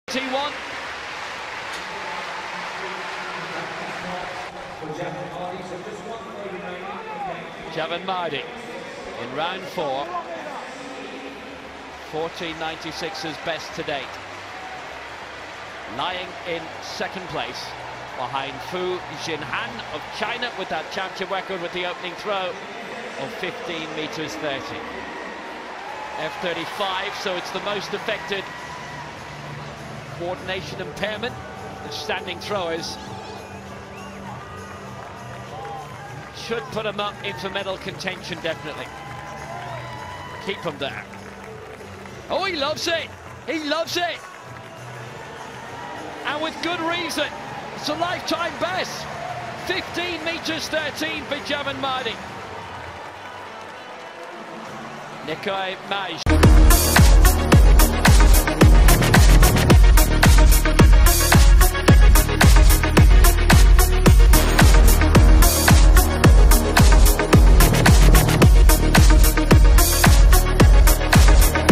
Seyed Aliasghar Javanmardi|Bronze Men's Shot Put F35 |Final| 2017 World Para Athletics Championships
Gold: Xinhan FU, China
Silver: Mehran NIKOEE MAJD, Iran
Bronze: Seyed Aliasghar JAVANMARDI, Iran

Start List:
Seyed Aliasghar JAVANMARDI, Iran
Hernan Emanuel URRA, Argentina
Sam RUDDOCK, Great Britain
Mehran NIKOEE MAJD, Iran
Edgars BERGS, Latvia
Xinhan FU, China
Abdulmhsen ALOTIBI, Kuwait
Tomasz PAULINSKI, Poland
Andrei VALENTIR, Moldova